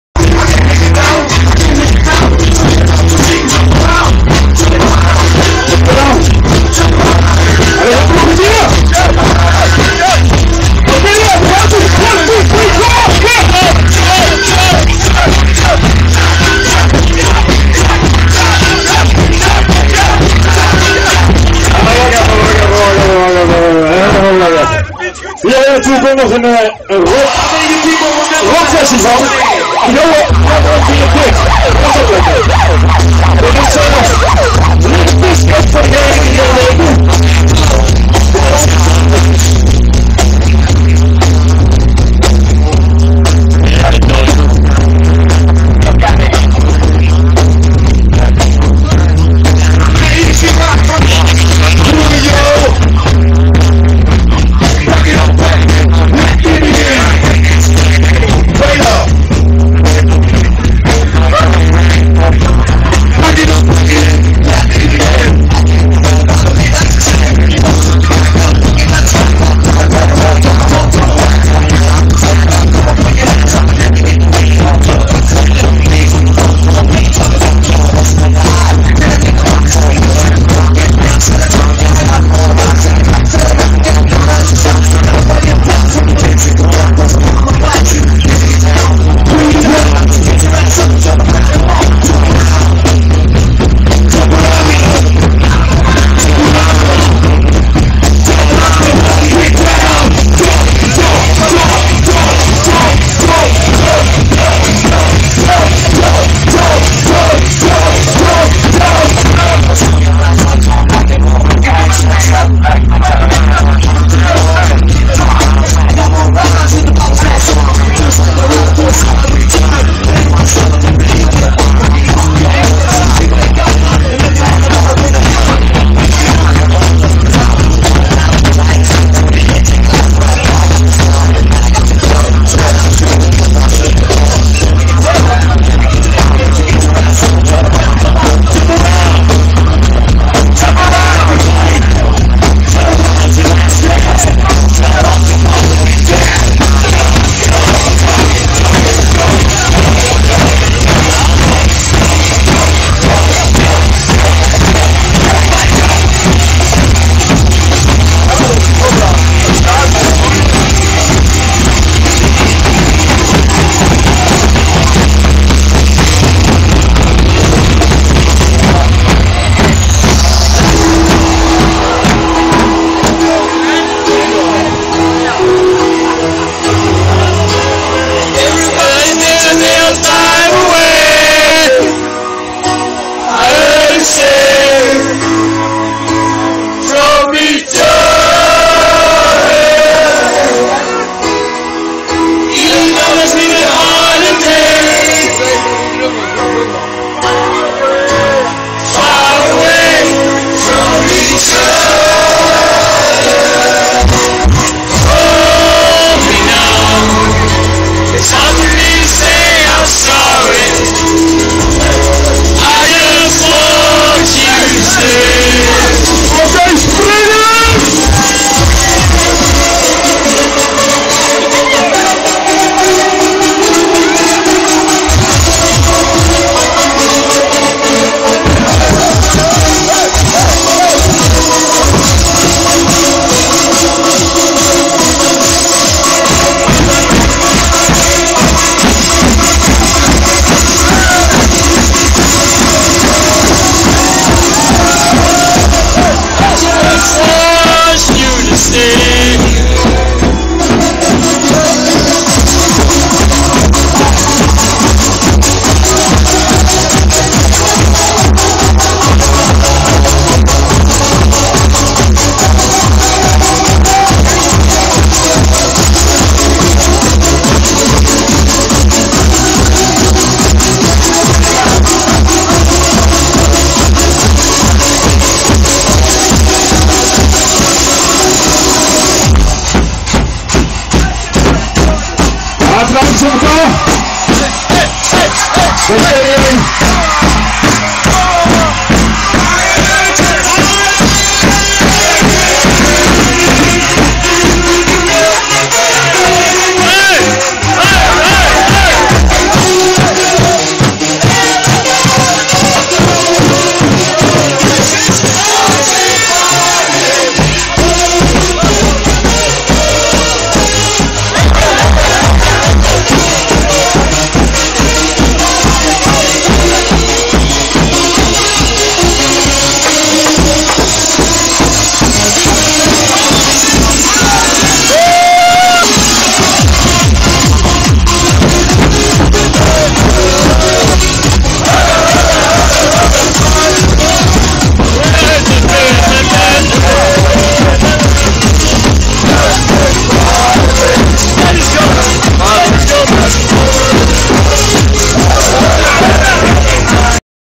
kampioensfeest Carvium.
Wat een geweldig volk. En let goed op de man met drum.